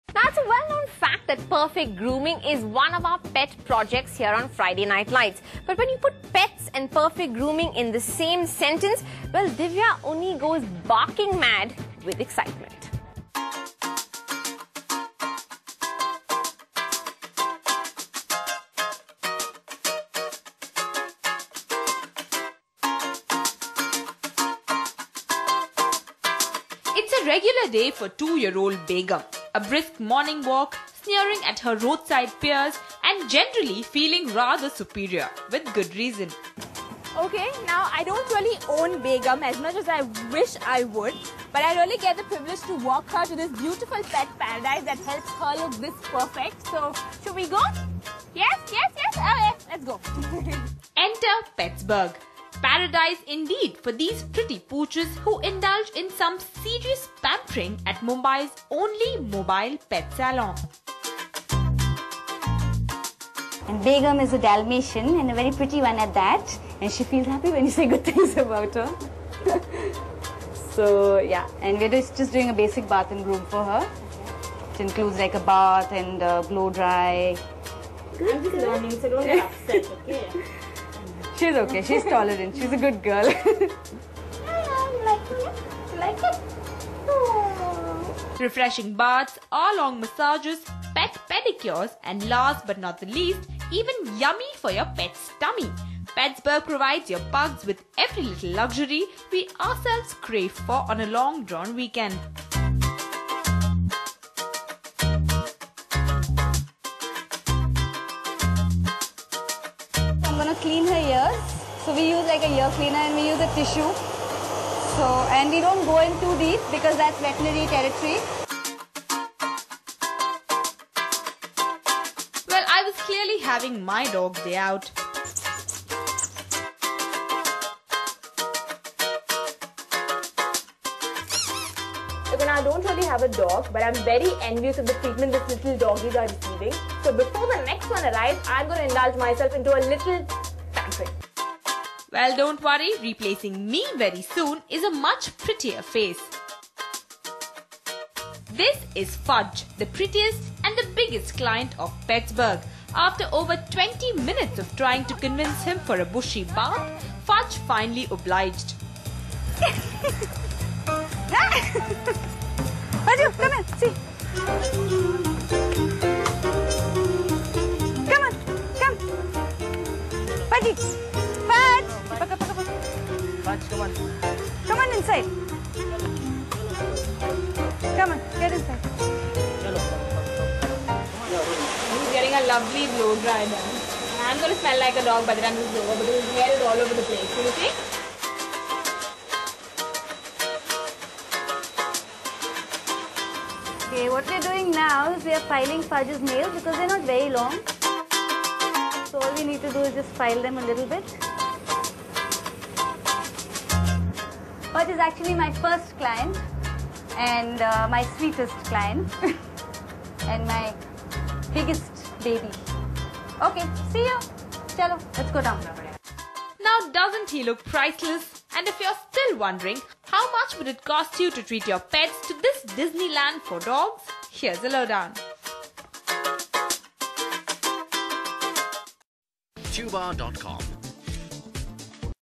Mumbai's only mobile pet salon
Petsburgh, Mumbai's only mobile pet salon, pampers pets and keeps them looking good.